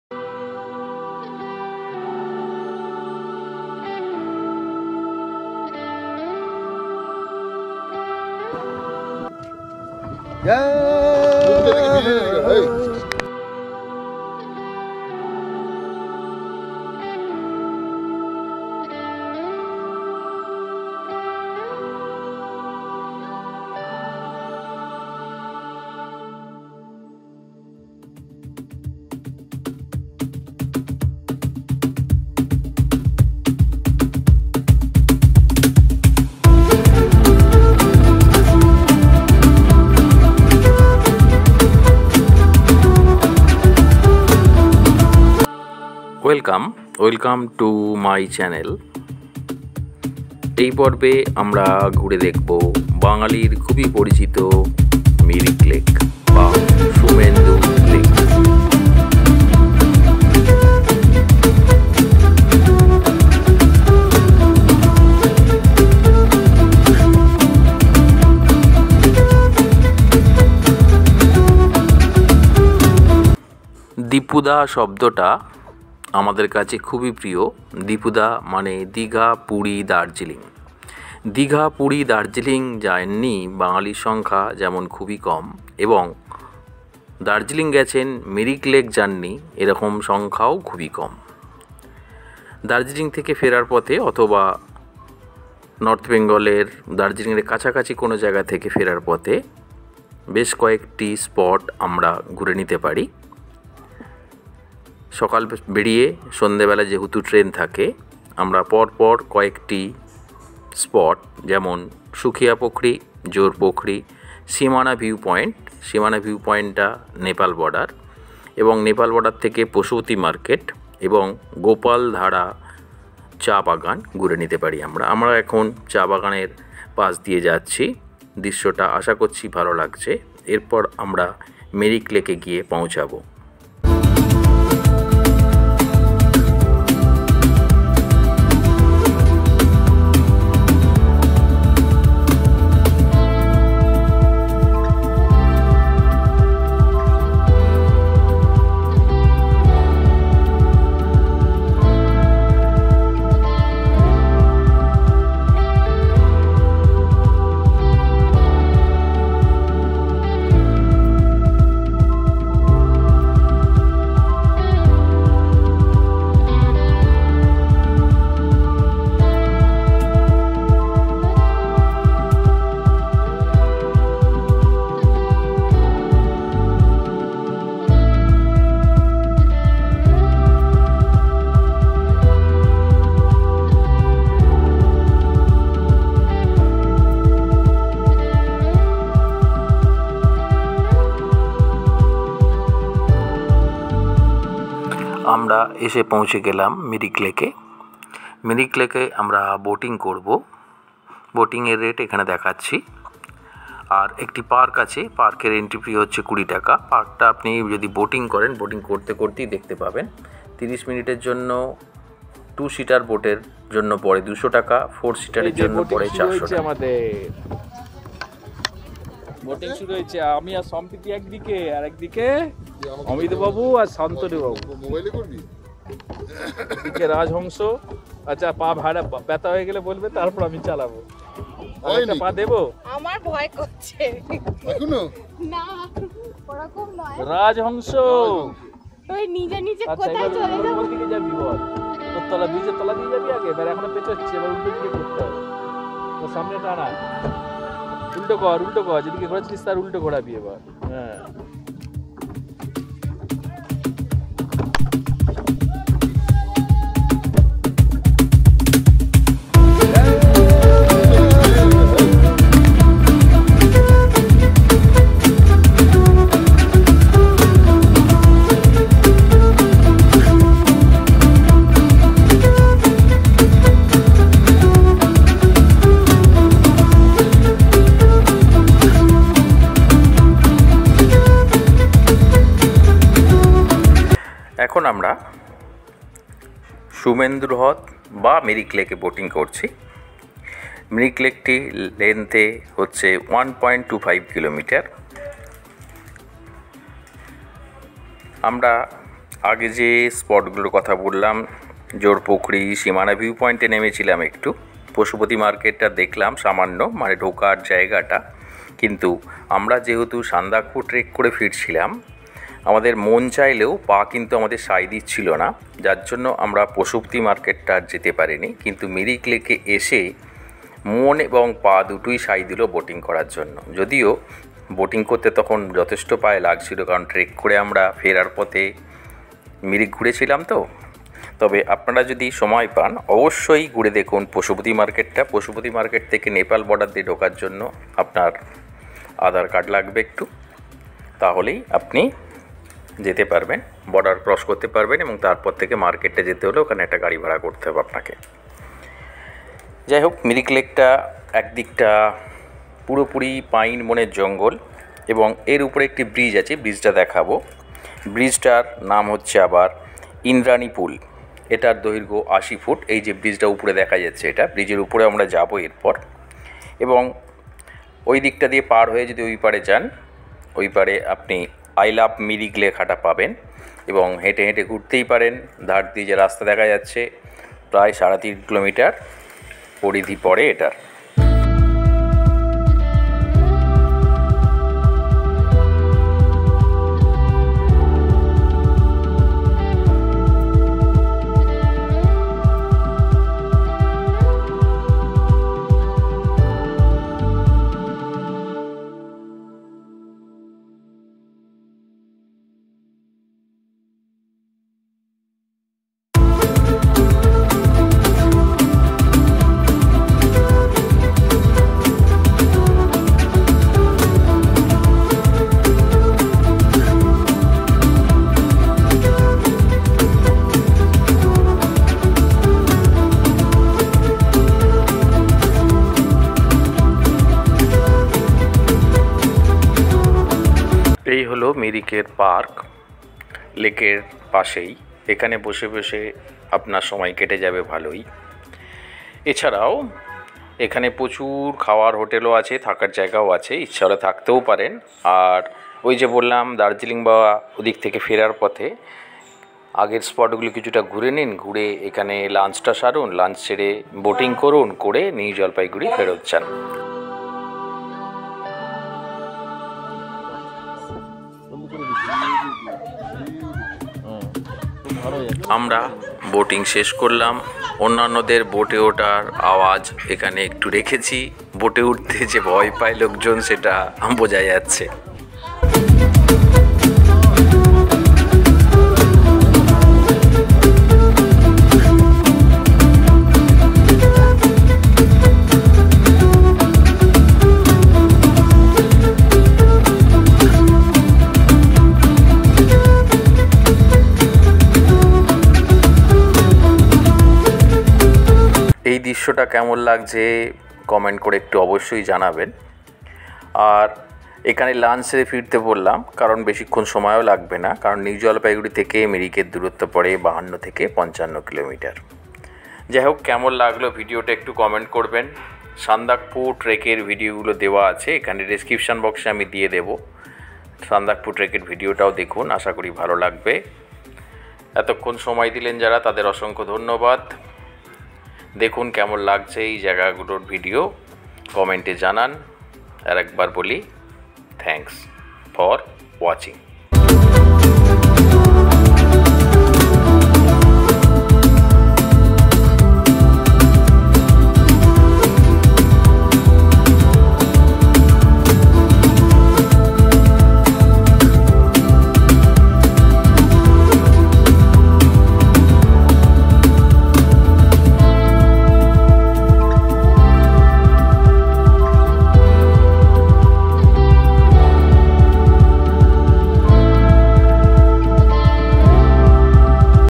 Mirik Lake.Sumendu Lake.Dhotrey to NJP.
Mirik Lake.  Dhotrey to NJP. পথে ছটি ভিউ পয়েন্ট পড়বে, সুখিয়াপখারী, jorpokhari, সীমানা ভিউ পয়েন্ট, নেপাল বর্ডার, পশুপতিনাথ মার্কেট এবং মিরিক lake। NJP to Mirik ৫৫ km.